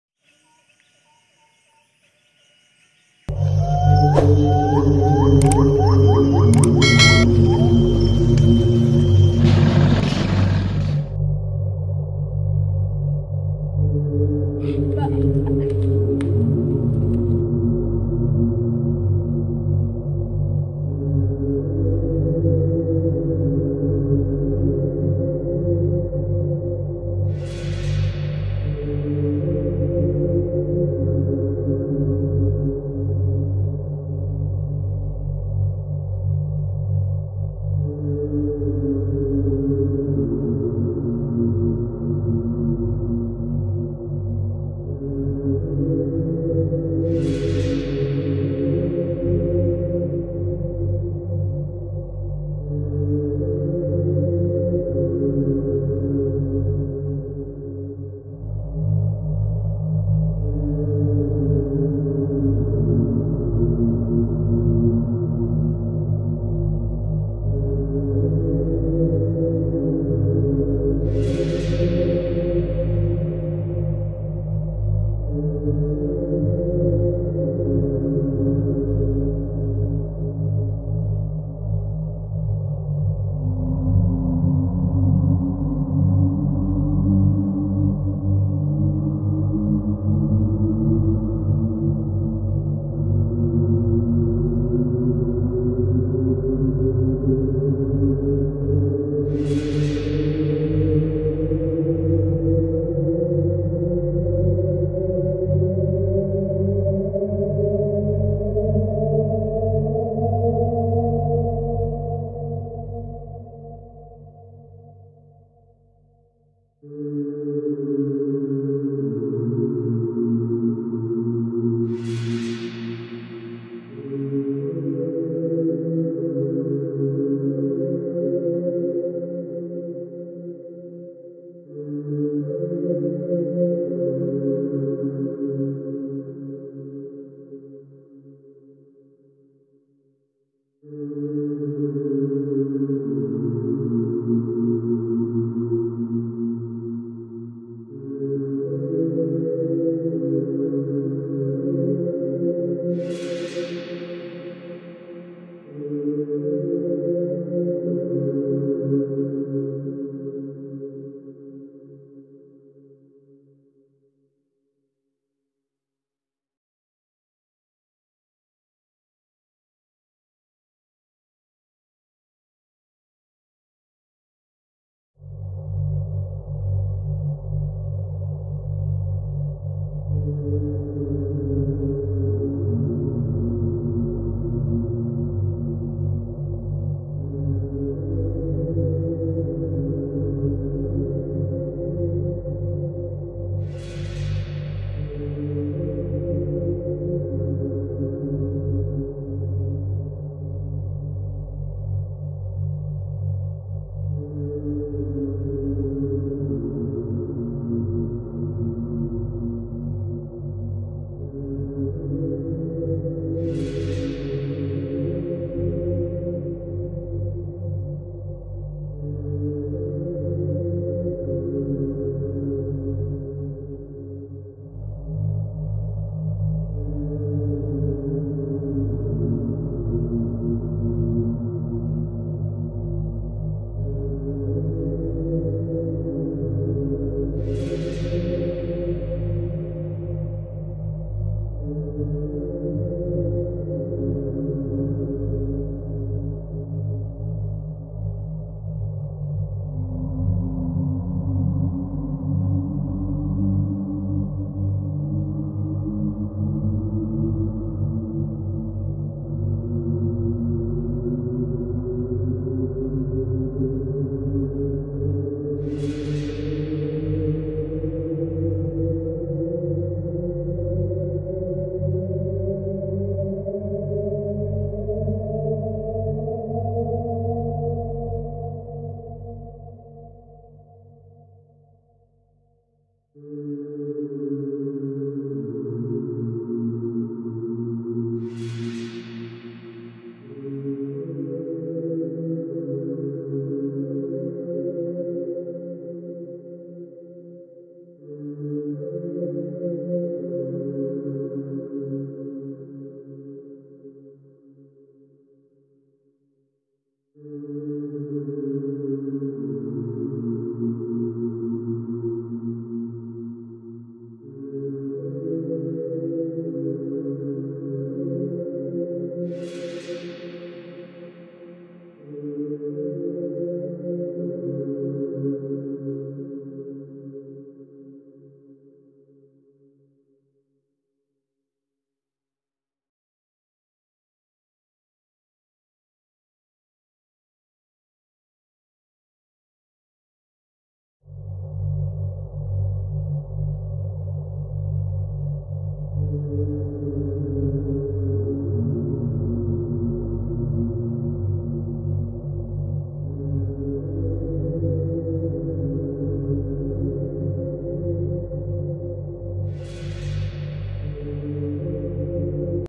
Bocah angon di Bawak kerumah suku mante
Saat penelusuran suku mante bocah angon ini di Bawak suku mante pulang ke rumah suku mante